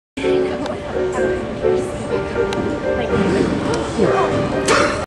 Abby;s Creative vault
Adirondack Classic March 7, 2010. Vault Blooper. She got a judges award for the most creative vault